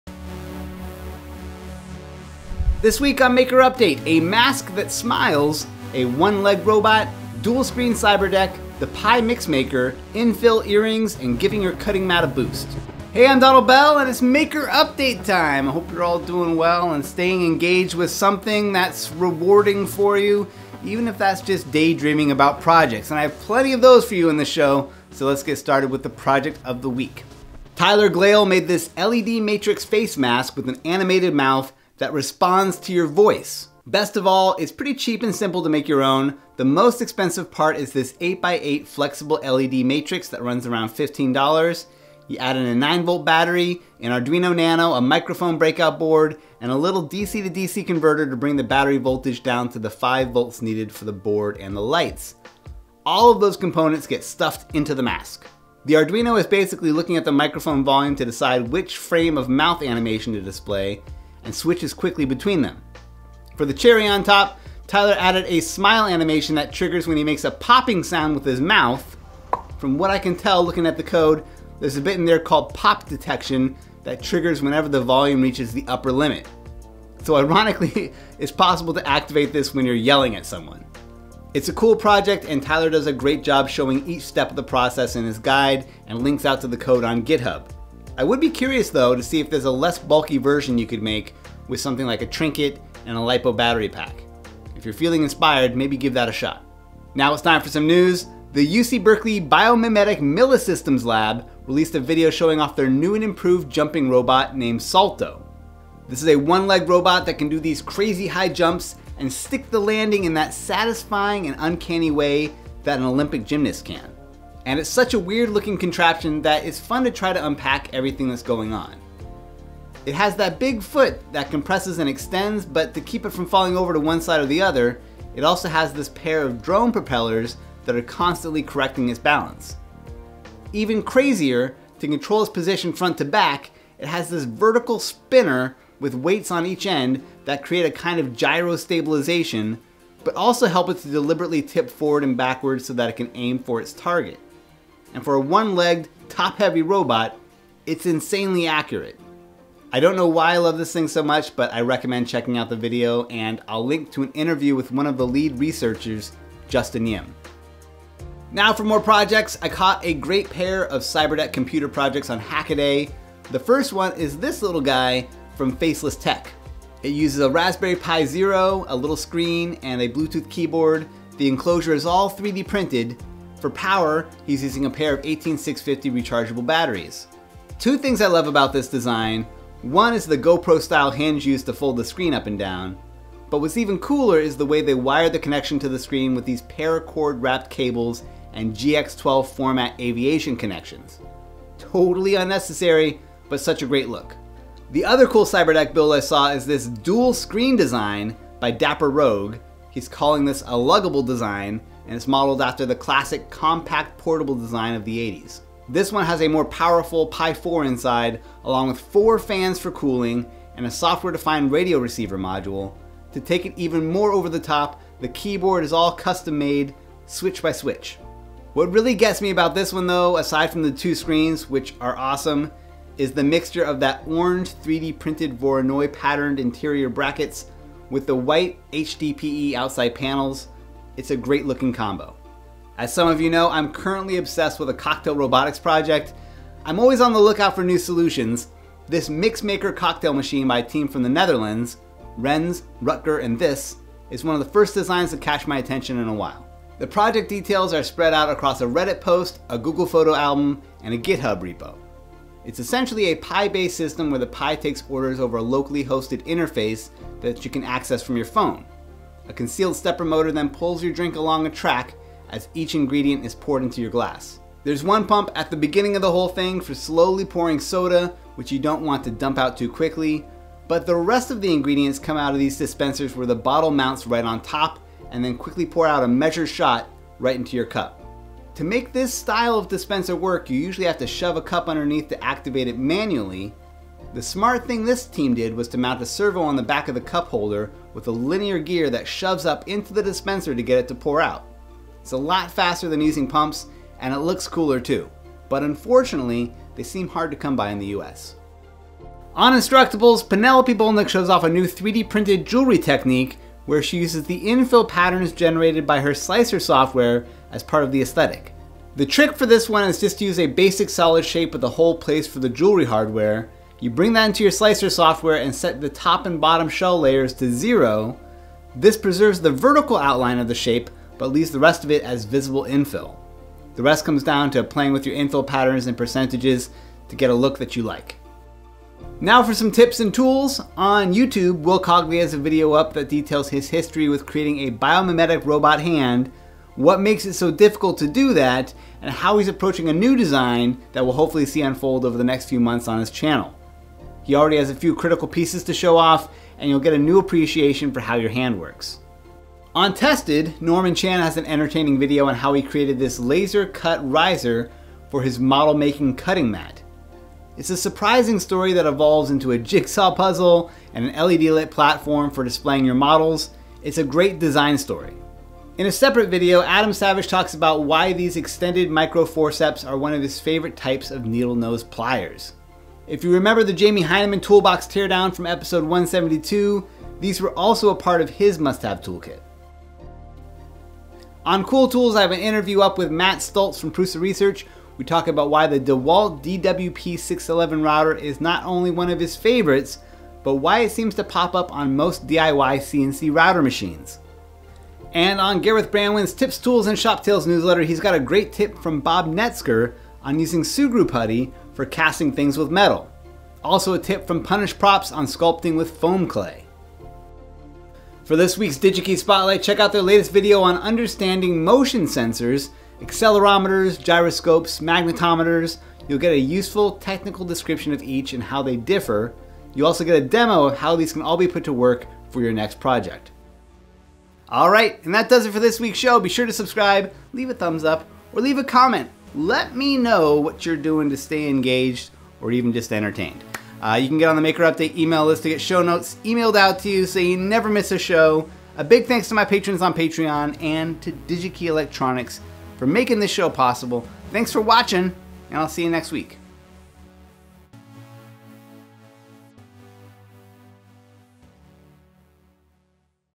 Grin and Wear It [Maker Update #181]
This week on Maker Update, a mask that smiles, a one-legged robot, dual-screen cyberdeck, the Pi MixMakr, infill earrings, and giving your cutting mat a boost.

Never miss a Maker Update! Sign up for the email newsletter (w/bonus links).

How to make a voice activated LED facemask by Tyler Glaiel

-=News=-

-=More Projects=-

-=Tips & Tools=-

Cutting Mat Riser and Lighting Platform by Norman Chan

Adam Savage's Favorite Tools: Extended Grabber Micro Forceps!

-=Digi-Key Spotlight=-